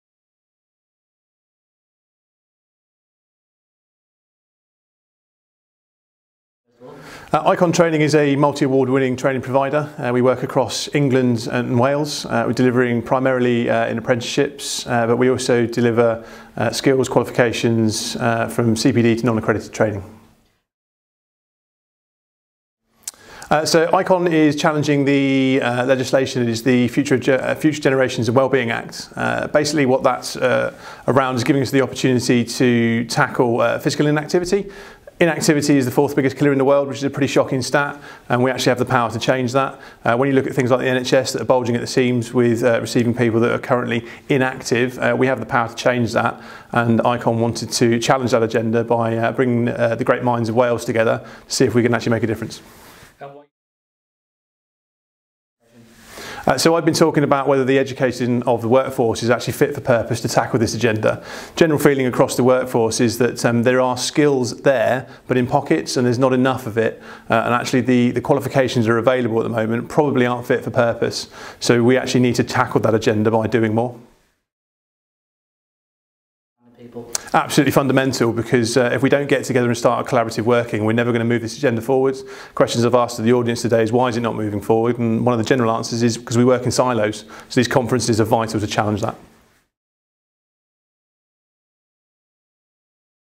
Gary Denton GettingWalesMoving interview
This video is about Gary Denton being interviewed at Why Sports Getting Wales Moving conference 2018

After his presentation at Why Sports Getting Wales Moving conference 2018 Gary gives an interview on the importance of these conferences, what he believes in and how Wales can move forward in terms of physical activity.